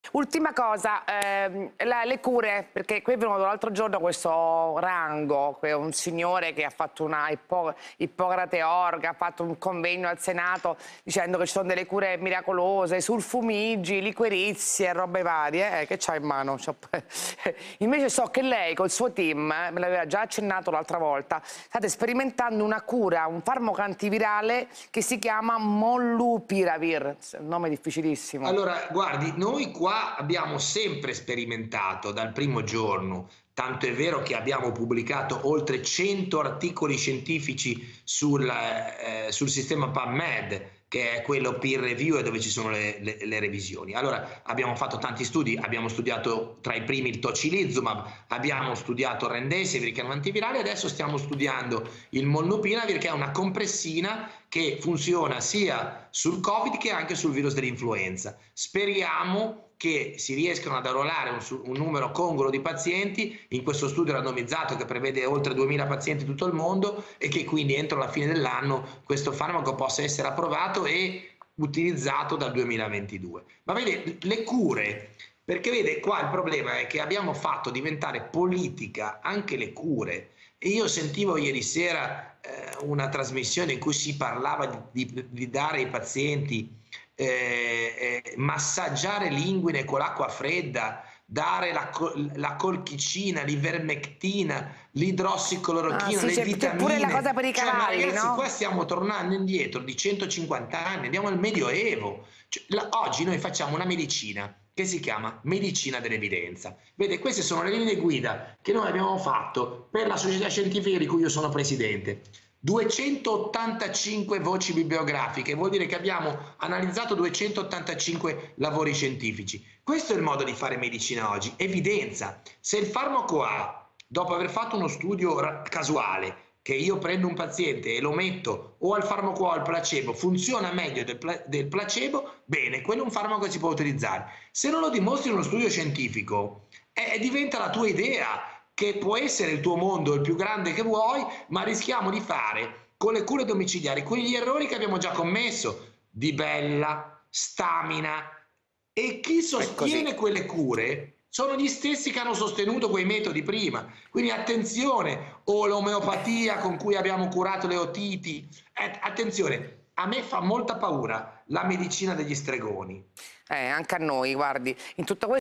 Molnupiravir nuova arma contro il Covid, Matteo Bassetti: "Mi fa paura la medicina degli ...
Matteo Bassetti (infettivologo): "Abbiamo sperimentato farmaci dal primo giorno, abbiamo pubblicato oltre cento articoli scientifici. Ora stiamo studiando il Molnupiravir, speriamo che si riescano ad arruolare un numero congruo di pazienti e che entro la fine dell'anno questo farmaco possa essere approvato".